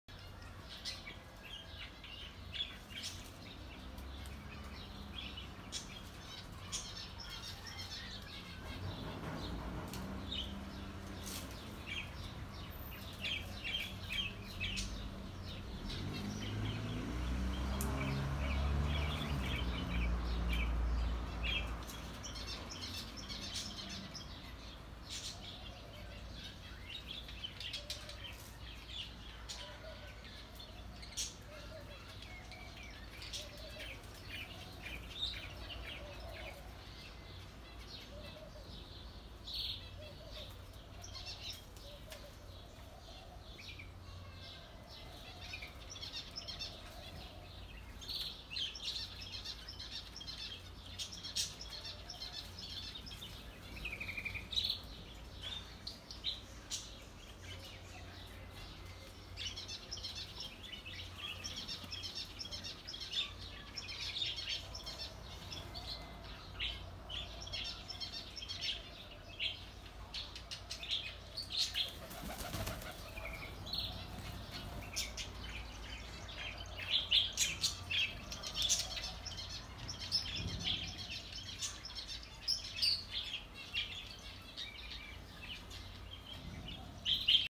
محمية للطيور في احدى الحدائق العراقية
تربية جماعية لانواع مختلفة من البلابل والكناري وطيور الحب والكوكتيل والجاوا والهدهد ويور اخرى داخل حديقة البيت